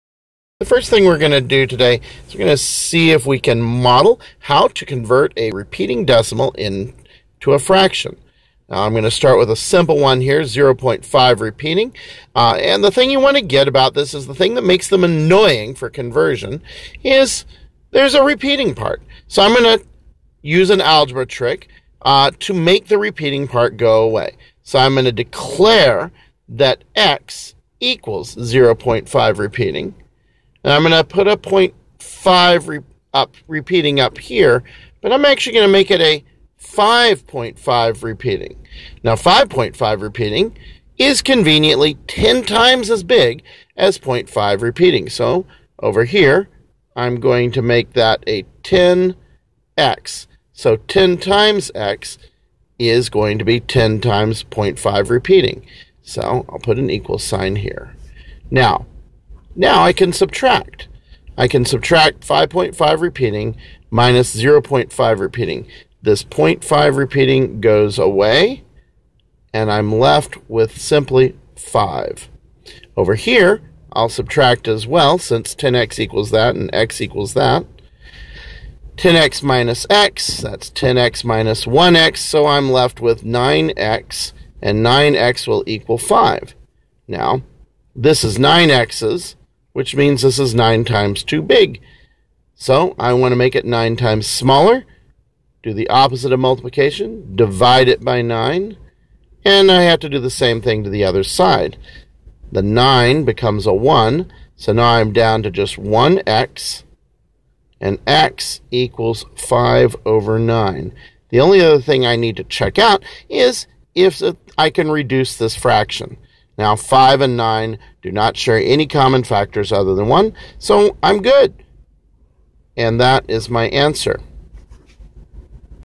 Converting a Repeating Decimal to a Fraction 1: 0.555.....
In which I model how to convert a simple repeating decimal into a fraction.